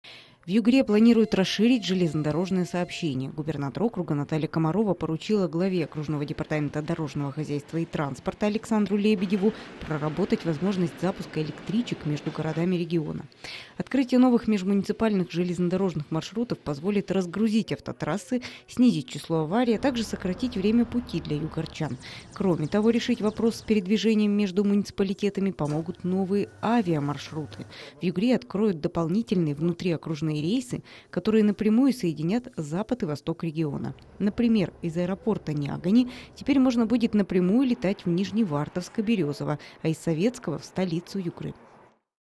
В Югре откроют новые межмуниципальные авиа- и ж/д-маршруты
В Югре планируют расширить железнодорожное сообщение. Губернатор округа Наталья Комарова поручила главе окружного департамента дорожного хозяйства и транспорта Александру Лебедеву проработать возможность запуска электричек между городами региона.